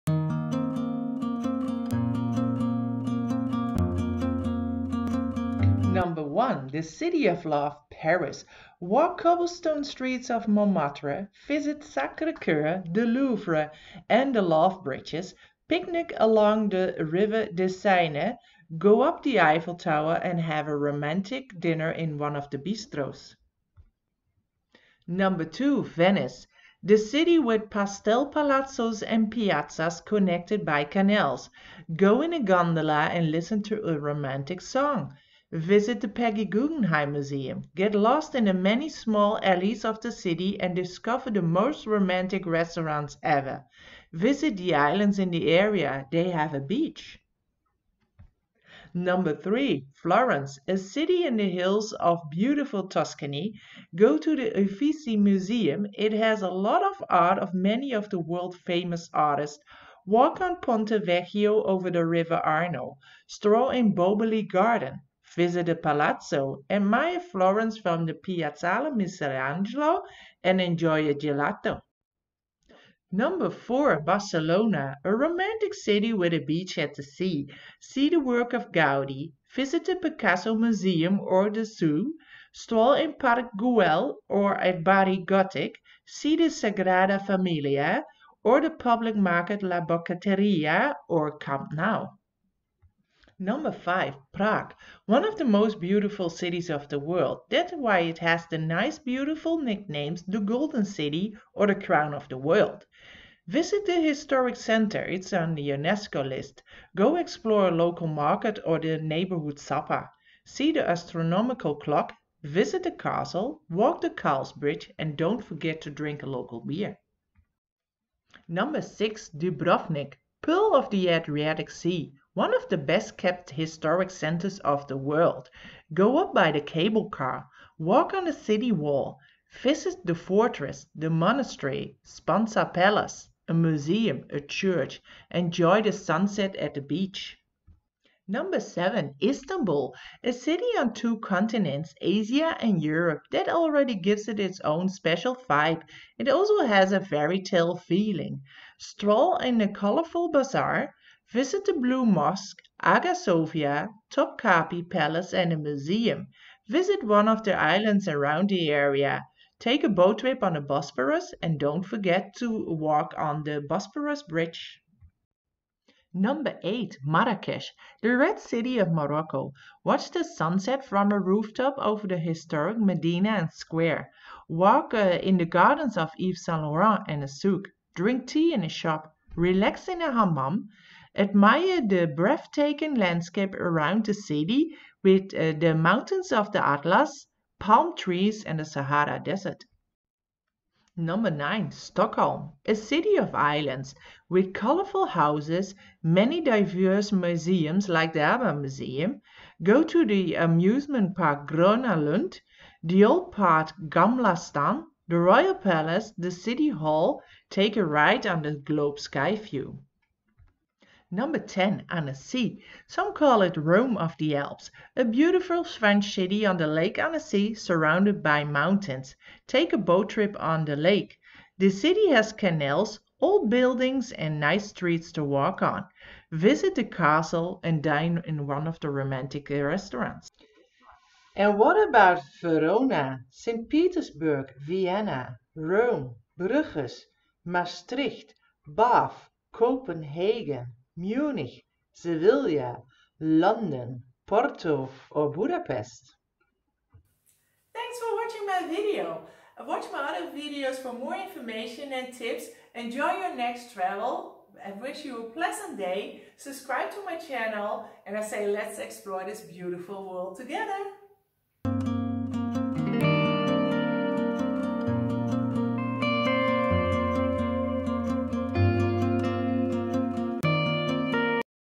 What are the Most romantic cities in Europe. 10- tips plus10 extra
Romance is in the air. Anywhere and anytime. And yet there are a number of cities in the world that add a little to the romance. Visit these cities with your loved one for Valentine's Day, your wedding proposal, to celebrate an anniversary, for a birthday or just to celebrate love.

Smile universal language